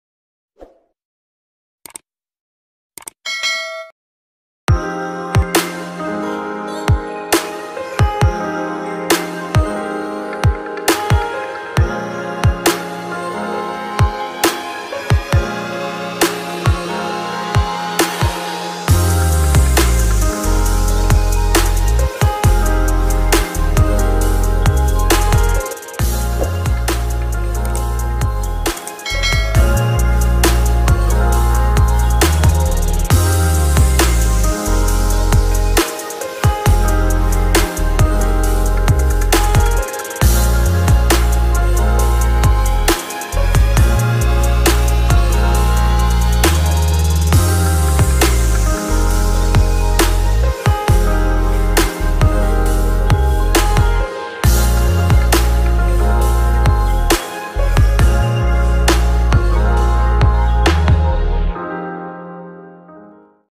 ഒരു ഗ്ലാസ്സ് BOOST ആയല്ലോ😊 I BOOST കലക്കിയത് I ഇത്ര എളുപ്പം ആയിരുന്നോ ഇത്😜
BOOST കലക്കിയത് ഉണ്ടാക്കുന്നതാണ് ഈ വീഡിയോ

INCREDIENTS

പാൽ
BOOST
SUGAR